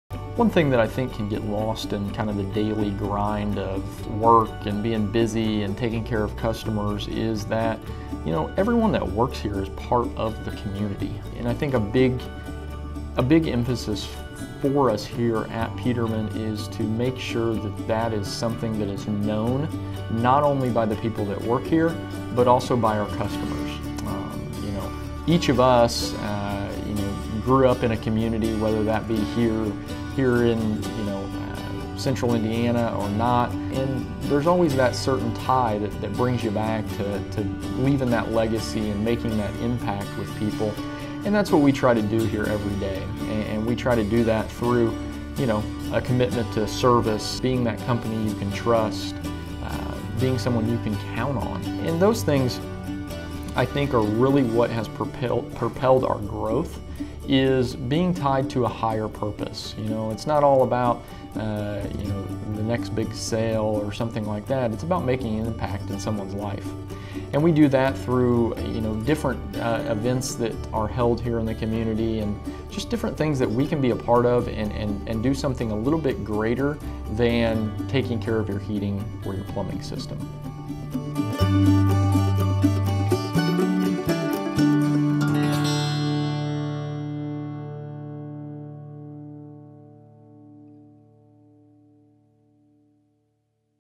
More Then a Company, a Community
We love being a part of the Indiana community, and we love taking care of your home. Check out today's video to hear Chad talk about why we try to be more then a company, we want to be a positive force wherever we are.